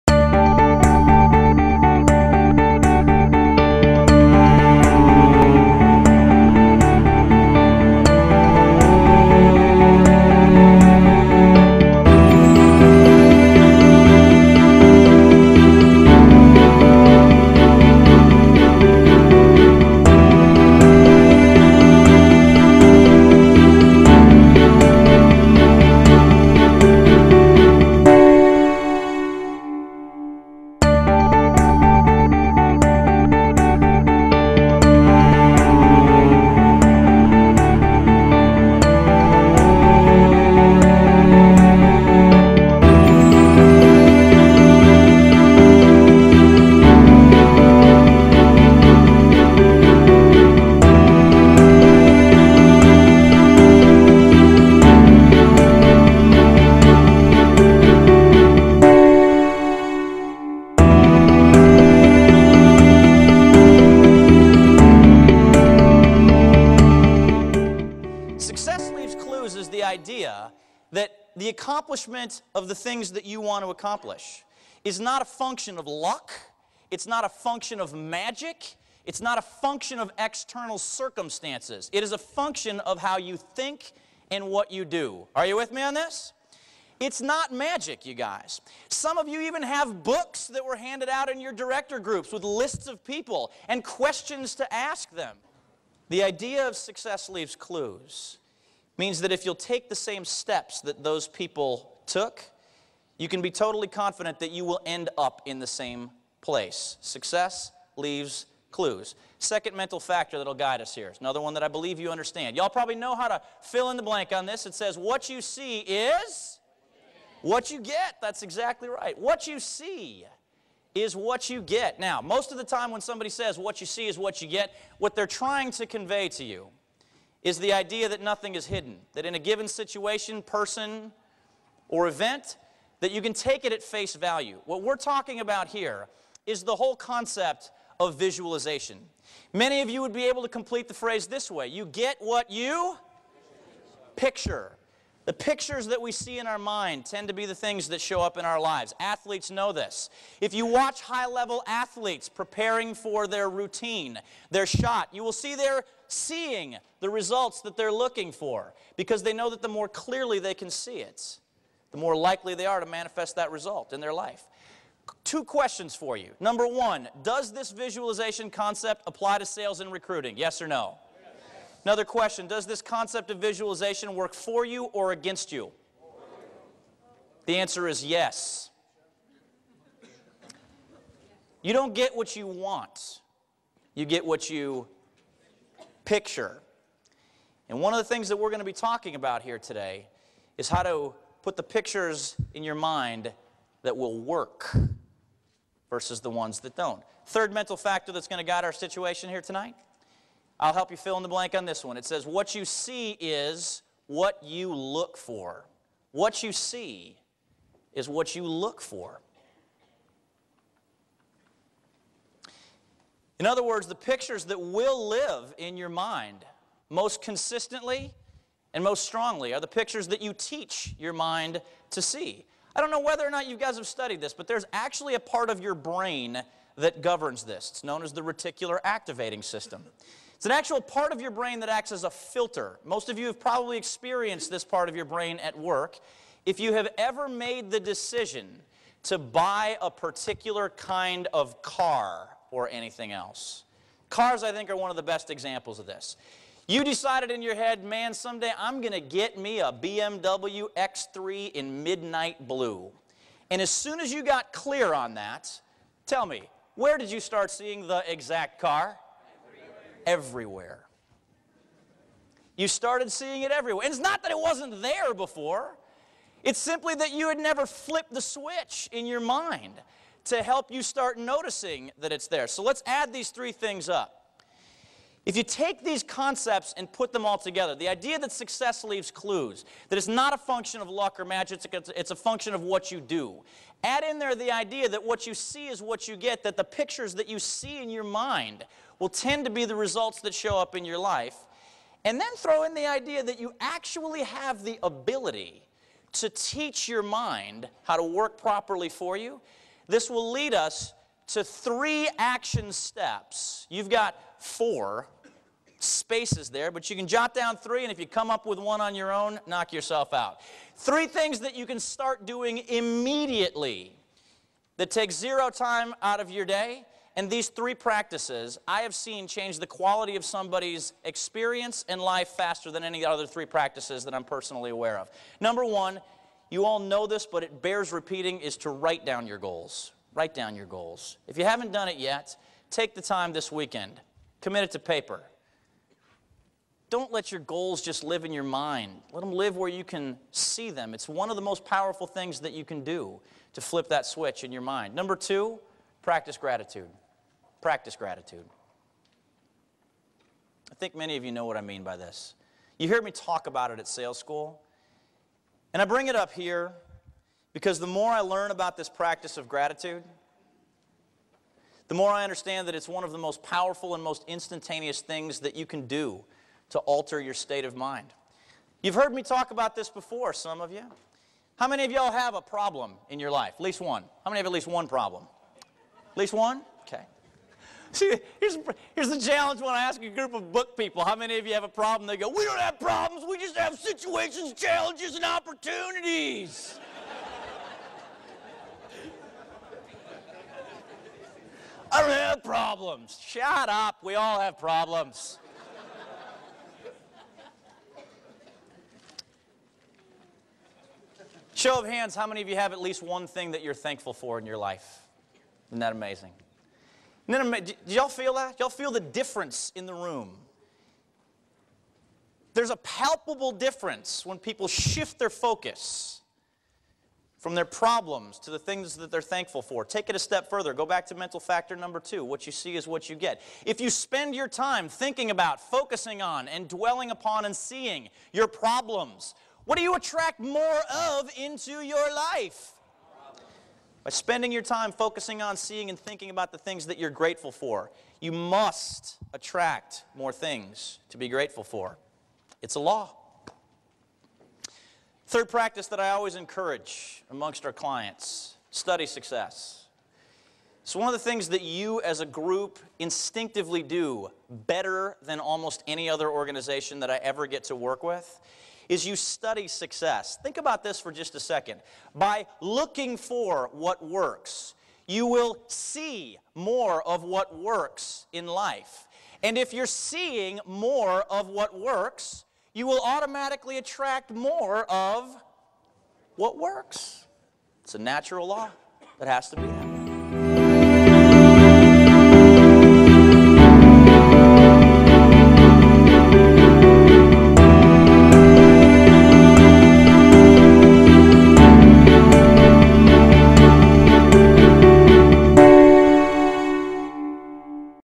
How to Break Records - Roger Seip
This video features highlights from Roger Seip's four-hour program, "How to Break Records" and is also offered as a keynote program.

How to Break Records

Records were meant to be broken.  Are you ready to break a few of your own?

Motivation and goal setting skills give you the direction needed to channel your energy to achieve your "personal best" in any aspect of your life.

In How to Break Records, you will learn to harness your mental power and achieve your personal best in any area of life.

This program covers the:

- Three mental factors that will automatically move you towards your goals
- Four necessary components of any record breaking performance
- How success leaves clues -- and where to find them
- What the "Law of Attraction" is and the impact it has on your life
- How to "flip the switch" that triggers achievement

With this knowledge you'll leave your existing comfort zone and experience your own record-breaking performance.

Roger's clients have used this knowledge to:
- Break nearly every sales record known to man
- Create a net worth that warrants the phrase "wads of cash"
- Develop a body that makes your personal trainer envious
- Wake up smiling about the person next to them

After hearing him once you will understand why thousands of business professionals have turned to Roger Seip to take their dreams and make them a reality.